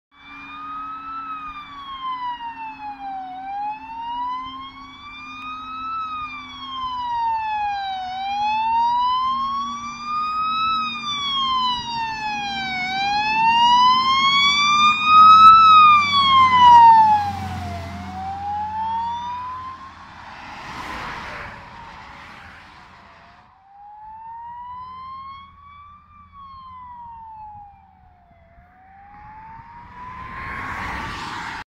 JRG ŚWIEBODZICE Alarmowo do zdarzenia 562[D]25
Zastęp z JRG Świebodzice udaję się alarmowo do pożaru w miejscowości Świny